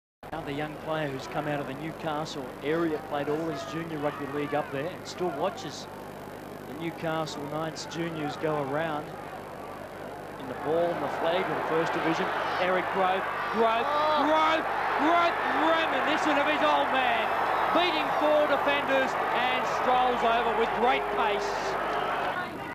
eric grothe jnr scores against the magpies 99
eric grothe jnr scores against the  wests magpies in 1999